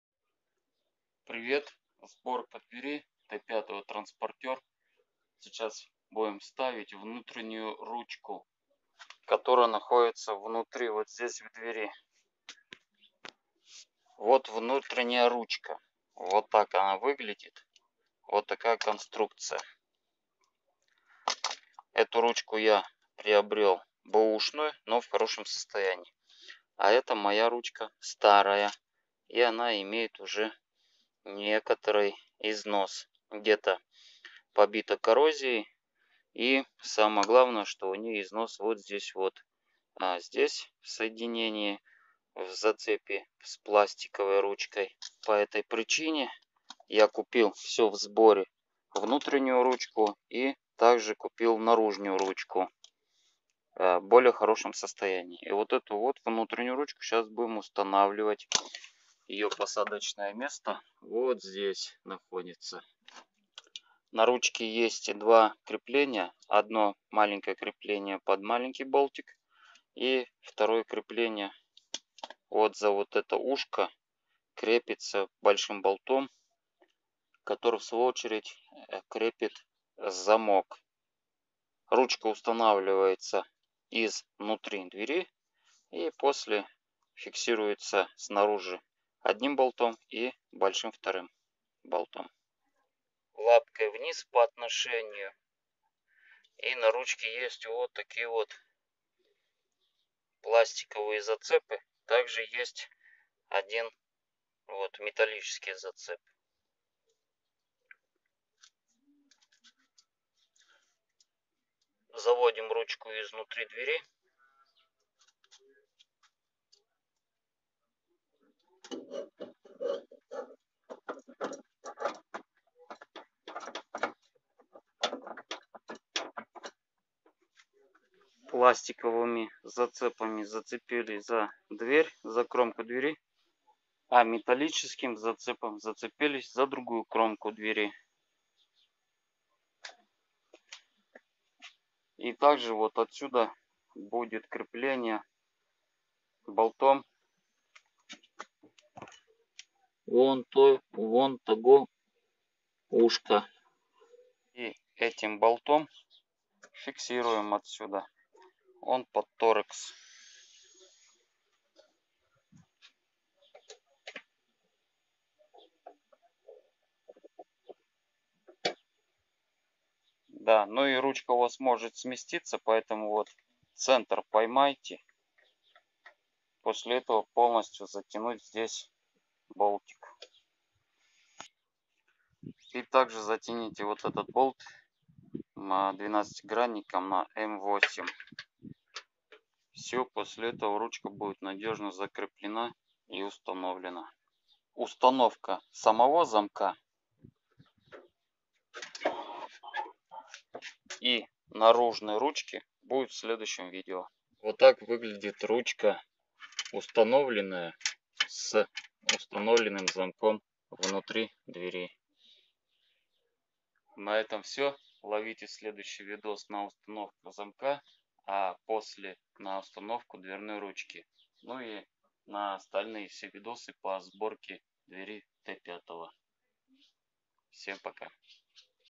Volkswagen T5 Как поставить Внутреннию ручку двери Правильно быстро ®️
КПД 78.  Т5 Transporter Как установить Внутреннию ручку двери Правильно быстро ®️